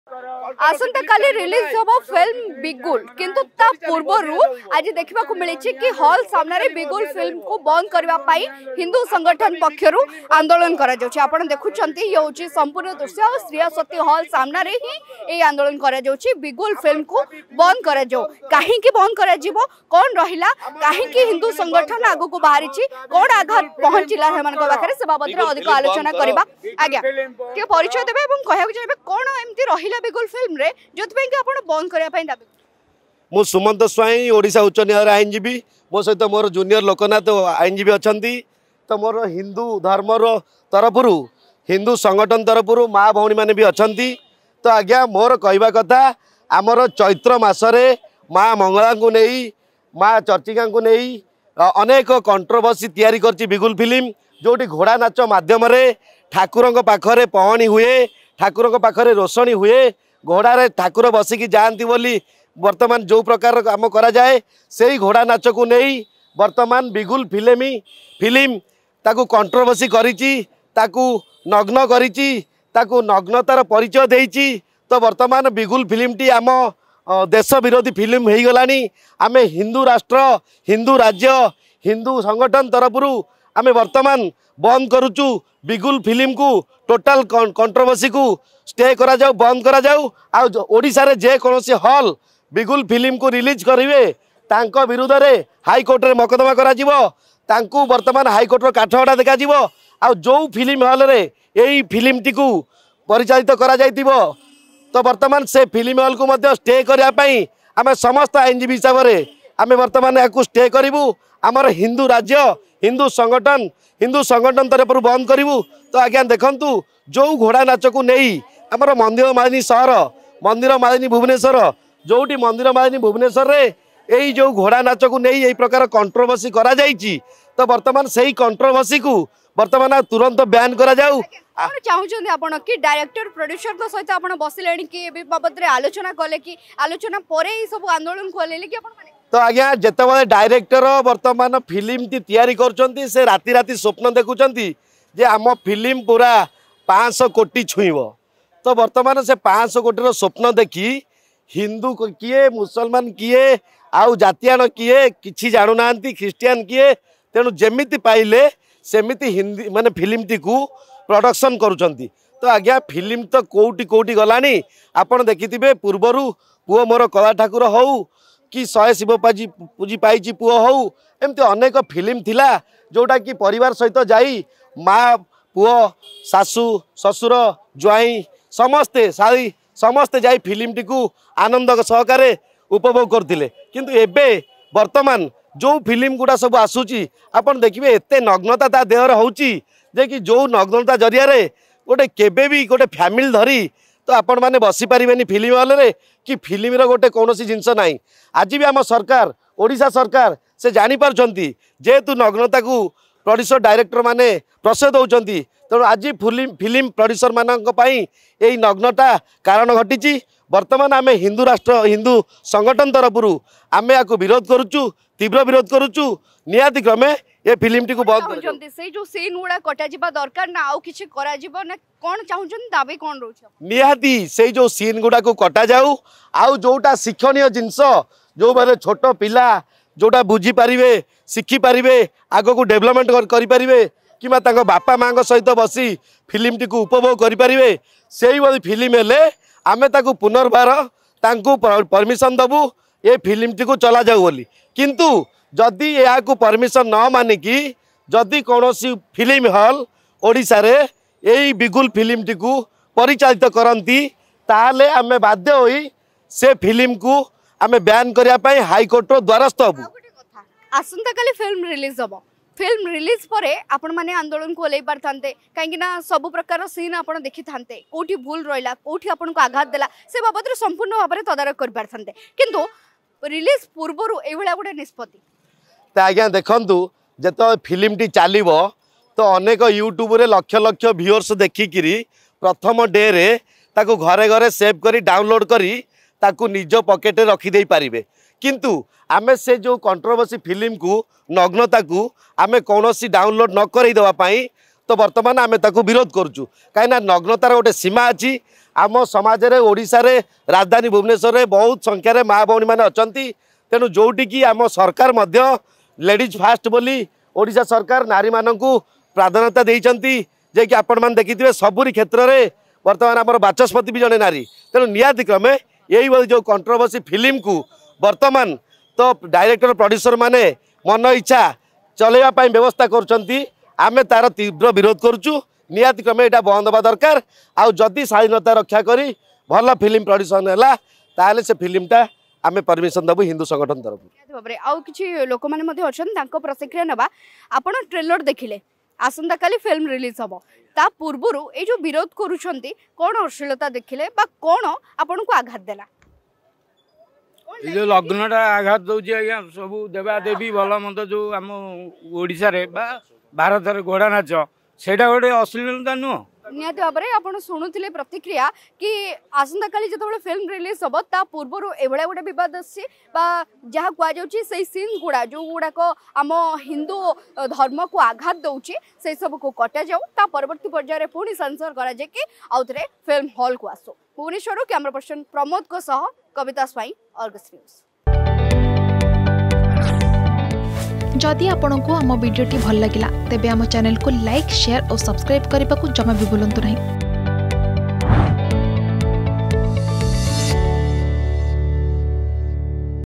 ବିଗୁଲ ଫିଲ୍ମକୁ ନେଇ ଦେଖା ଦେଇଛି ବିବାଦ || Film Bigul
ଆସନ୍ତାକାଲି ପରଦା କୁ ଆସିବ ଫିଲ୍ମ ବିଗୁଲ। କିନ୍ତୁ ରିଲିଜ୍ ପୂର୍ବରୁ ଫିଲ୍ମକୁ ନେଇ ଦେଖା ଦେଇଛି ବିବାଦ। ଫିଲ୍ମକୁ ବନ୍ଦ କରିବା ଦାବି ନେଇ ହଲ ସାମ୍ନାରେ ଆନ୍ଦୋଳନ କଲେ ହିନ୍ଦୁ ଧର୍ମ ସଂଗଠନ ।